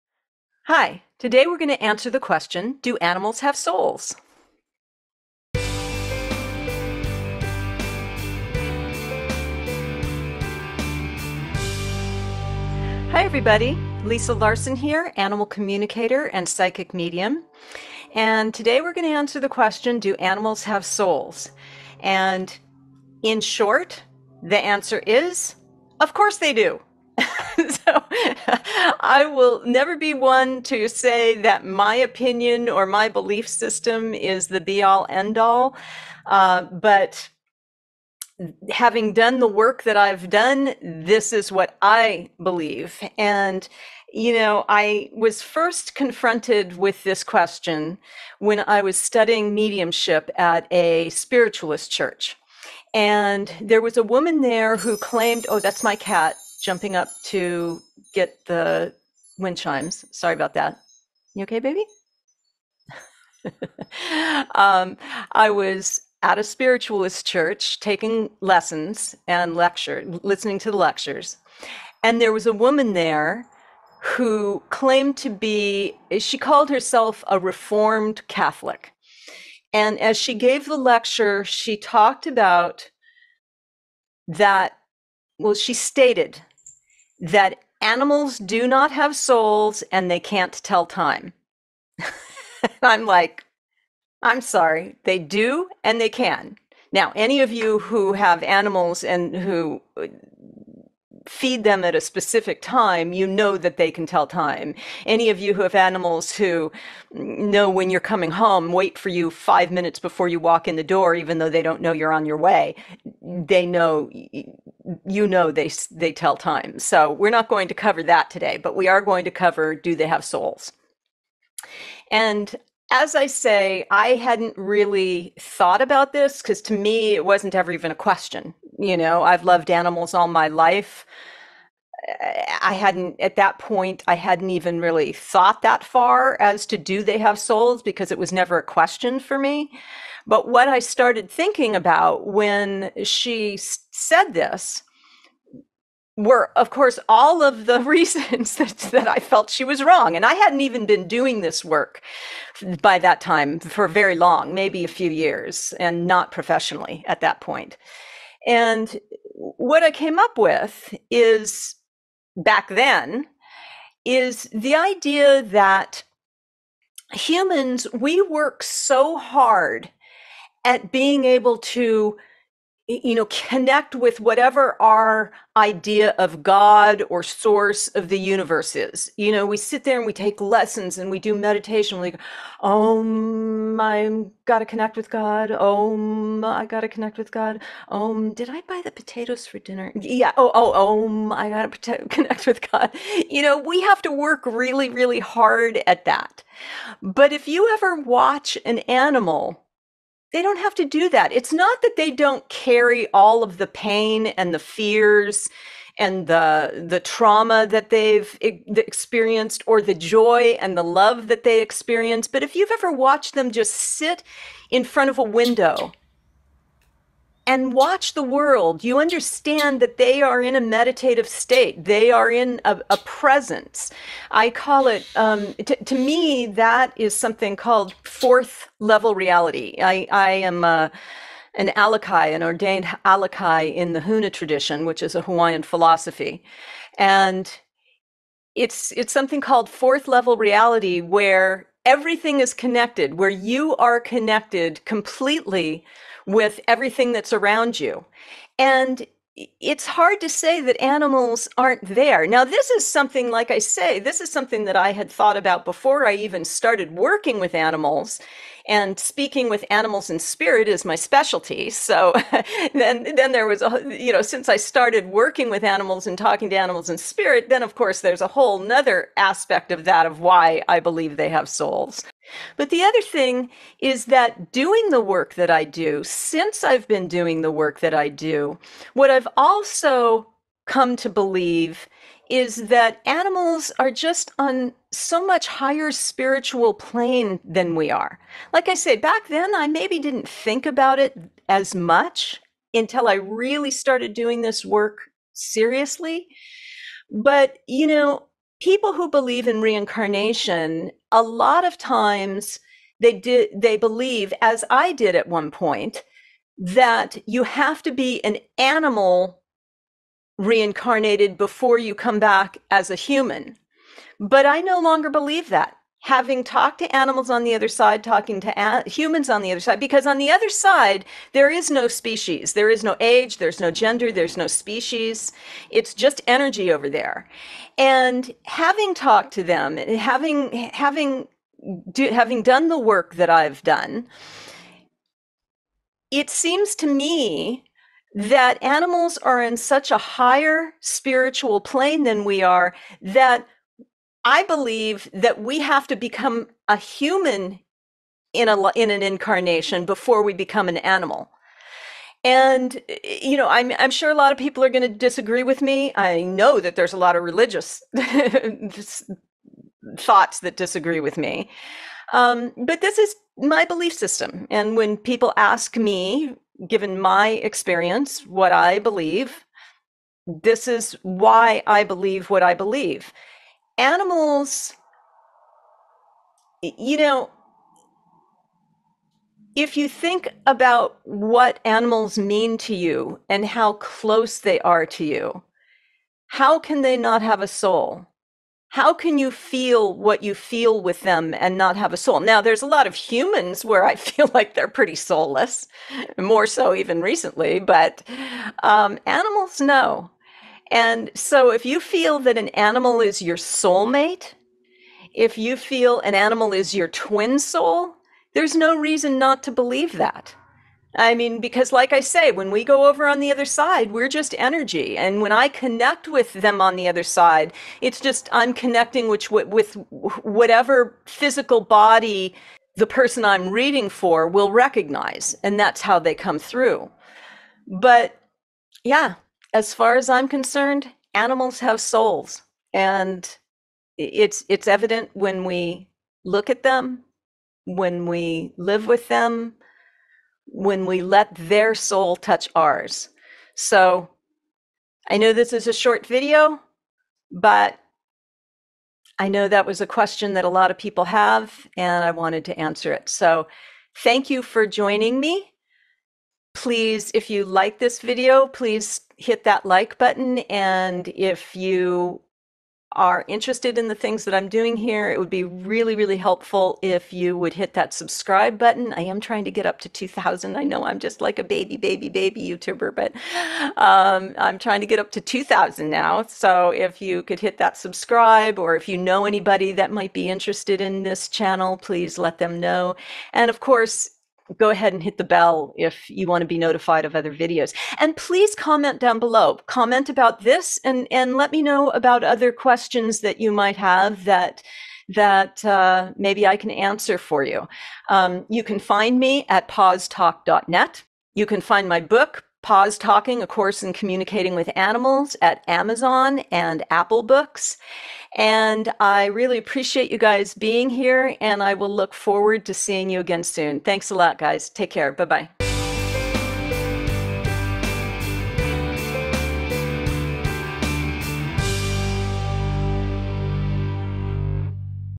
Soulful Companions: Answering the question, "Do Animals have Souls?"
"Unlock the mysteries of animal consciousness in our latest video! Delve into compelling evidence and diverse perspectives as we explore the profound question: Do Animals Have Souls? Join Lisa on a thought-provoking journey that sheds light on the spiritual connection between humans and the animal kingdom."

(In Disclosure:
You can help keep this video cast going by using the following monetized links. Thanks.) :)

Cat Toys:
Da Bird
Da Bee
Crinkle Tunnel